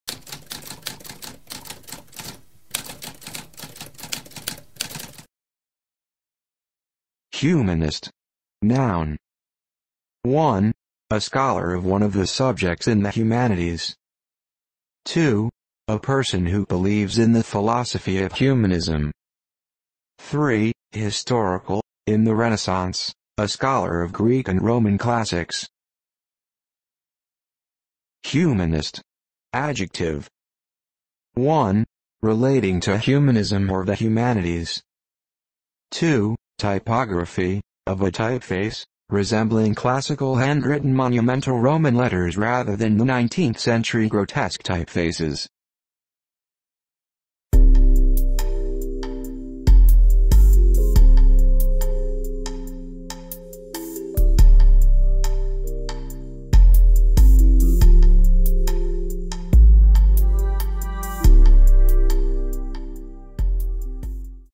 What does humanist mean?
A spoken definition of humanist.

Intro Sound:
Typewriter - Tamskp